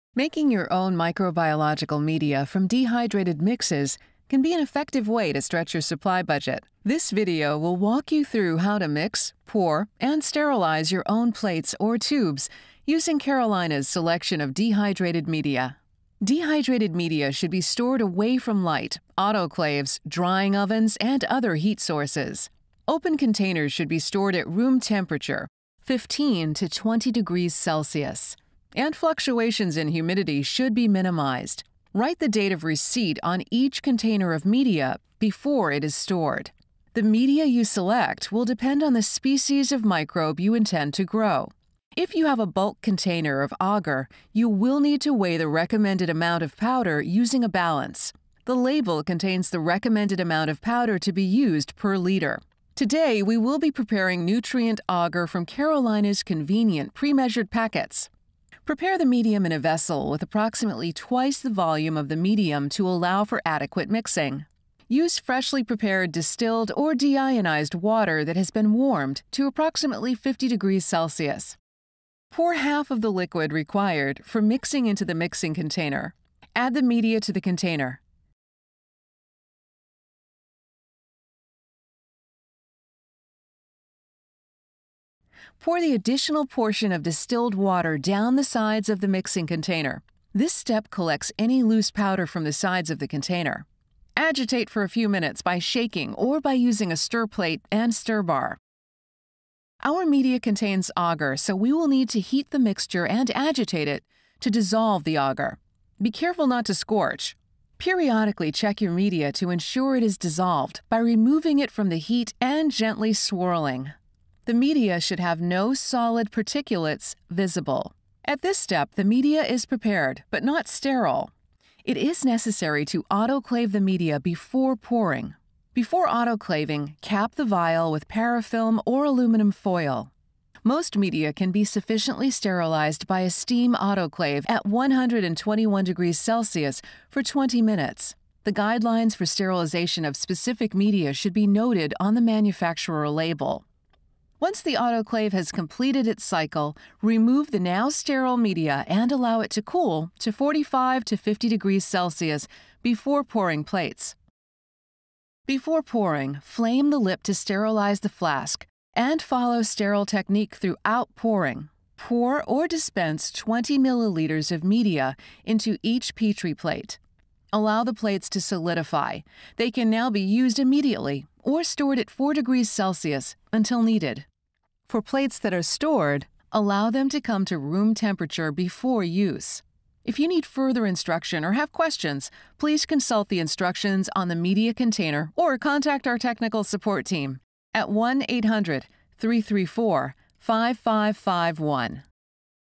Dehydrated Media Preparation
This video will walk to you through how to mix, pour and sterilize your own plates or tubes using Carolina’s selection of dehydrated media.